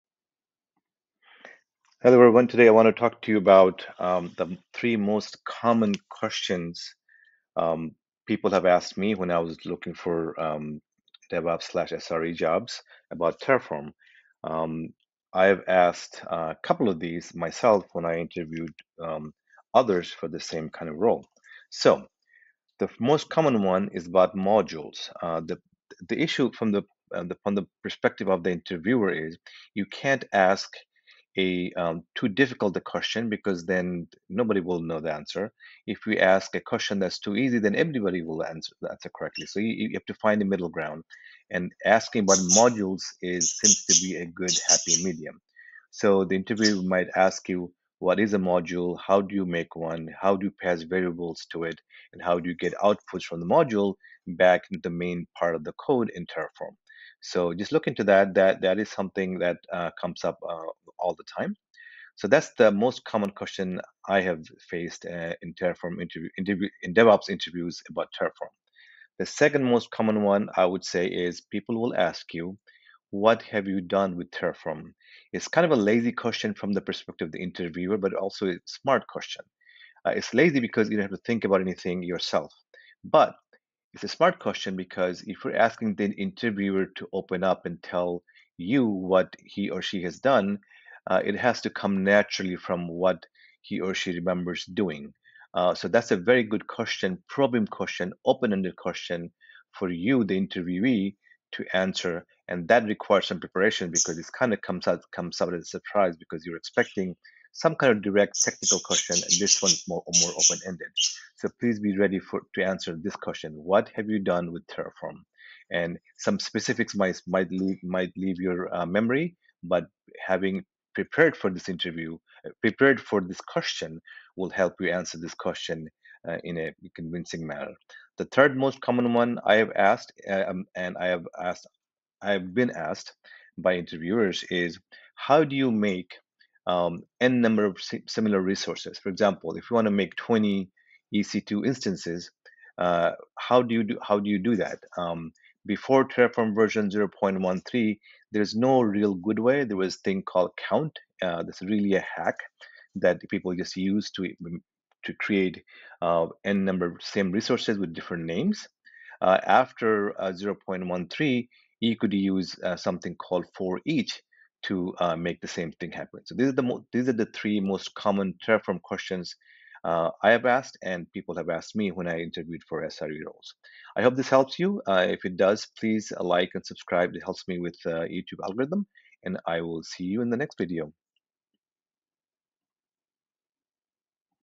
Top 3 Terraform Interview Questions I have been asked and I have asked
THREE MOST COMMON TERRAFORM INTERVIEW QUESTIONS

1. Module:
     what it is
     write one
     how pass variables to it and outputs back

2. What have you done with Terraform ?

3. How can you make N resources of the same type: e.g. 20 EC2 Instances